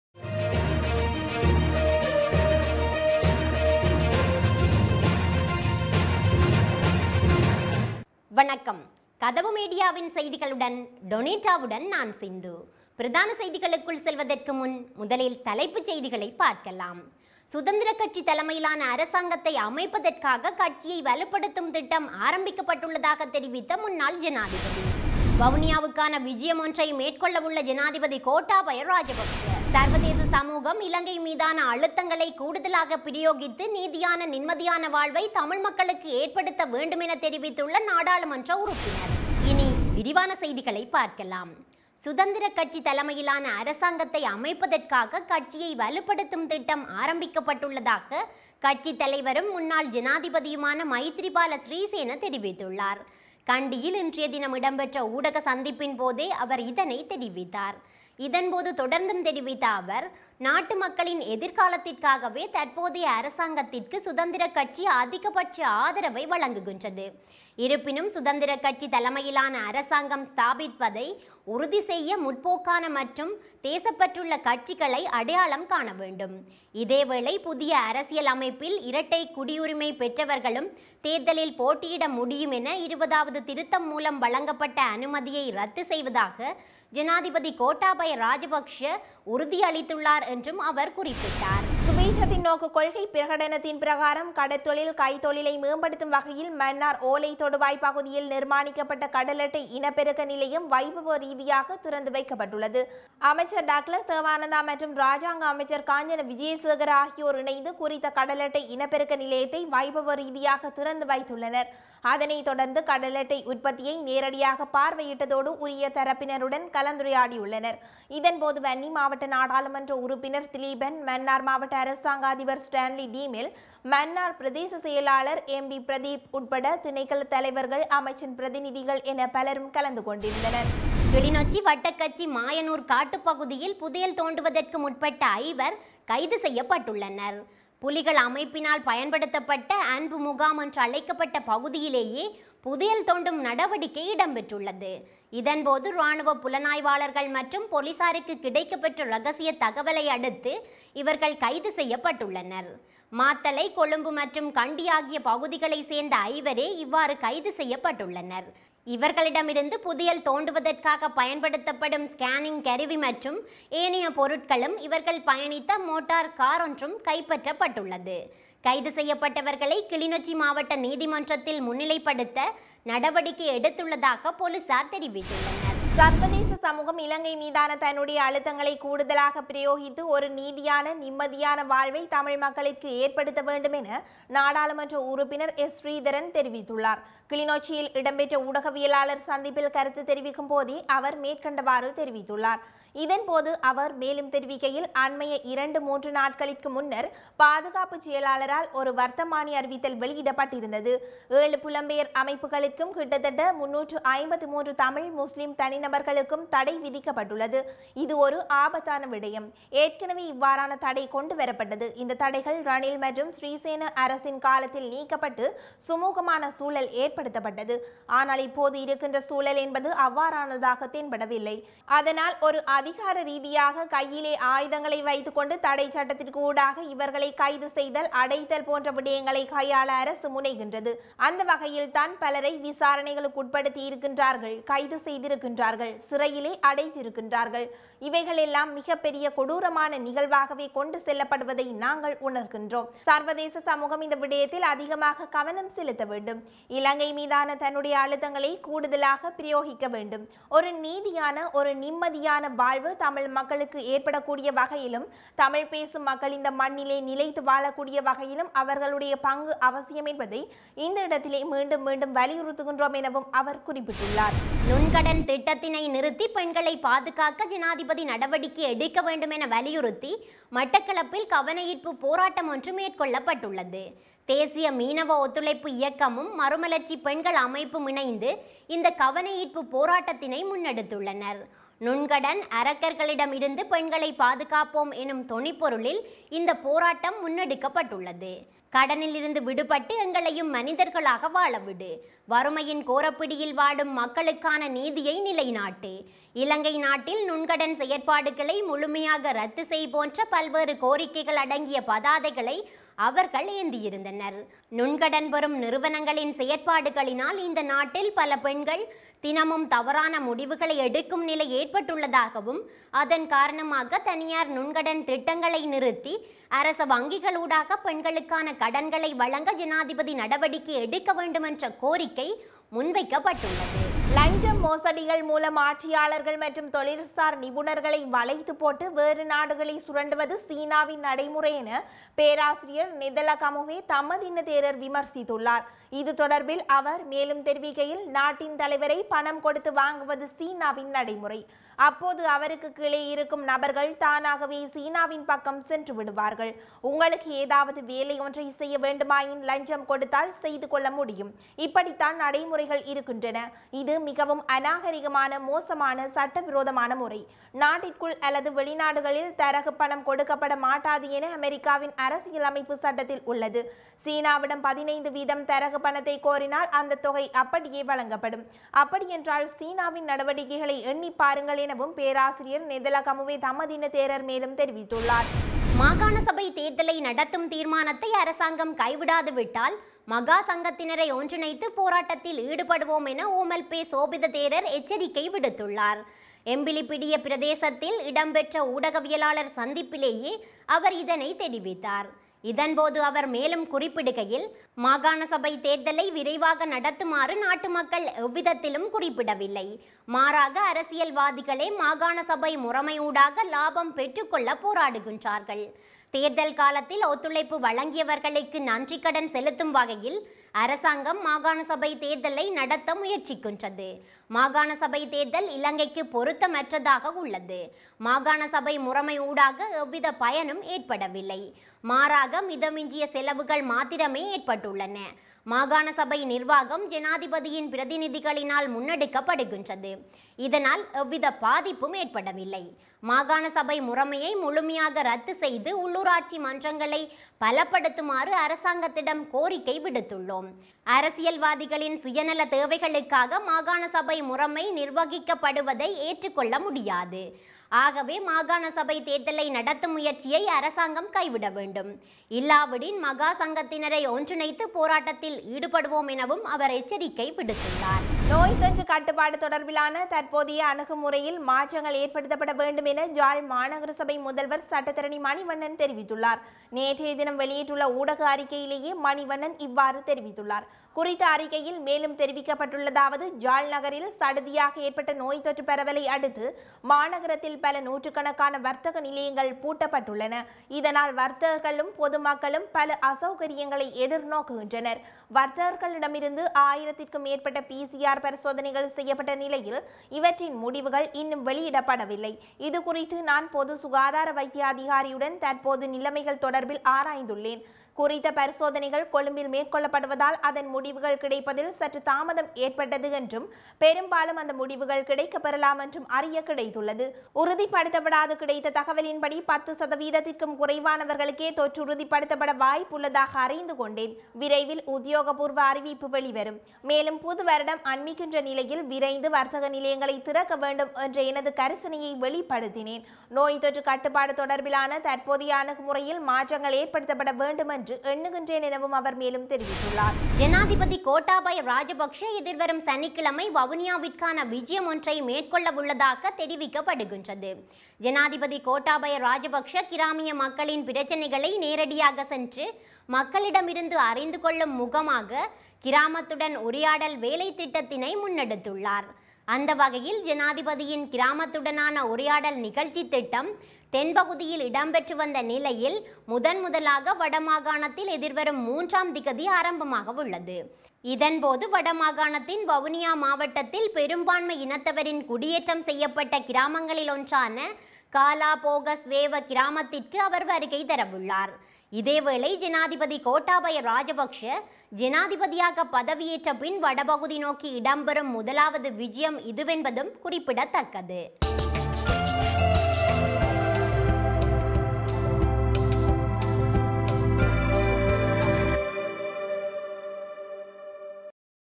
கதவு மீடியாவின் அதிகாலைச் செய்திகள் | Kathavu Media Early Morning News
The former president said a plan had been launched to strengthen the party to form a Freedom Party-led government. President Gotabhaya Rajapaksa to pay a visit to Vavuniya. The Member of Parliament has said that the international community should put more pressure on Sri Lanka to bring about a just and peaceful life for the Tamil people.

Our Sri Lanka news brings you most of the world current affairs, including Tamil politics and regional matters.